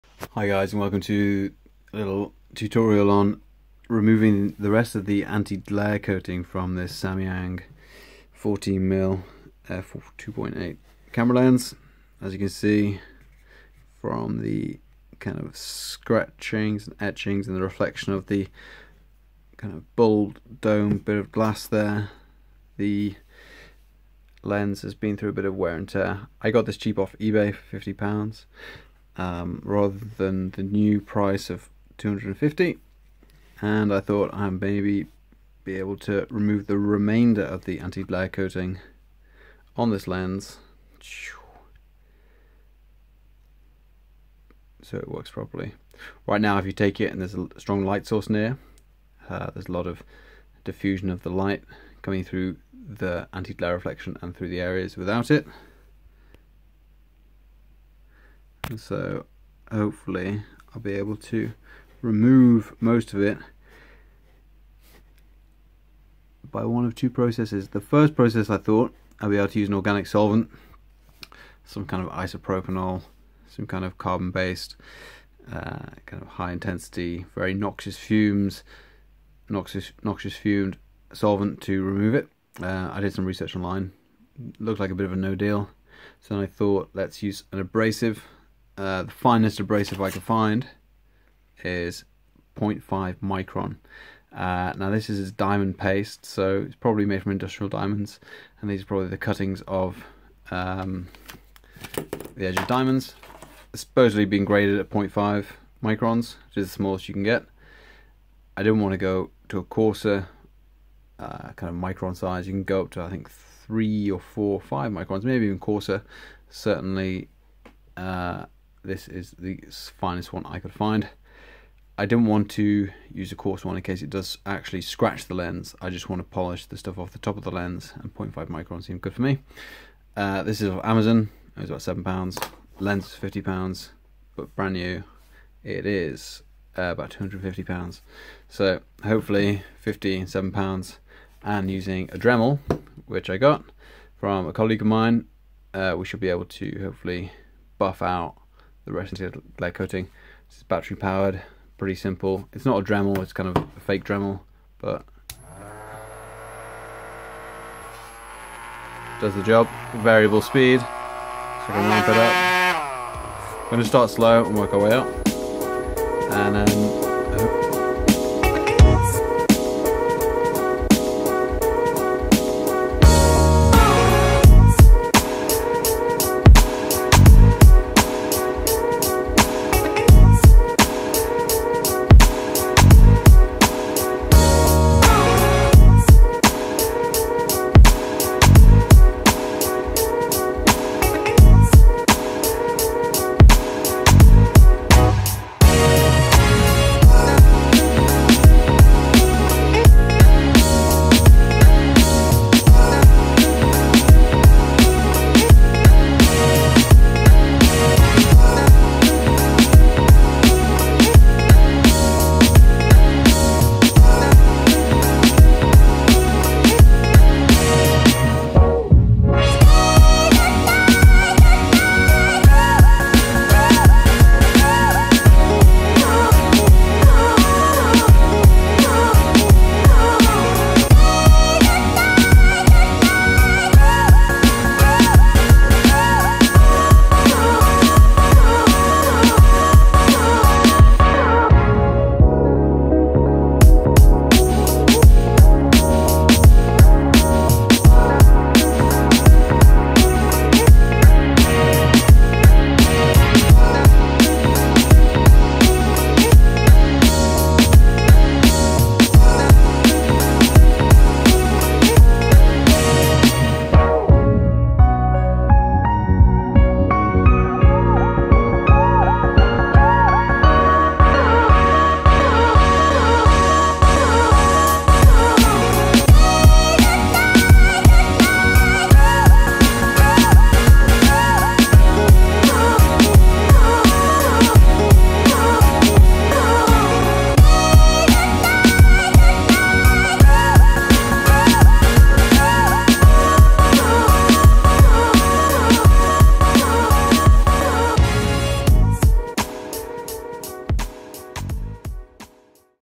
Anti-Glare coating removal with diamond abrasive. Samyang 14mm astro lens.
Removing a scratched anti-glare coating on a Samyang 14mm.

Abrasive polish of 0.5micron diamond paste and a dremel.